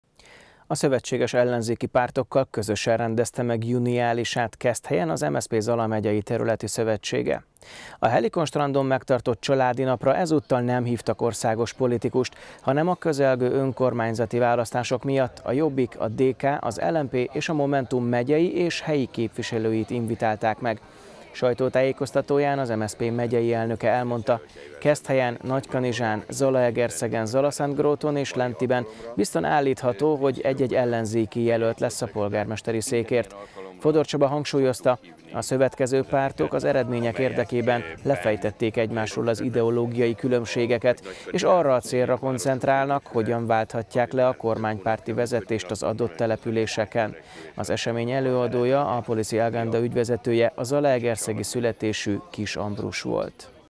Juniális a helyhatósági választások jegyében
A szövetséges ellenzéki pártokkal közösen rendezte meg juniálisát Keszthelyen MSZP Zala Megyei Területi Szövetsége.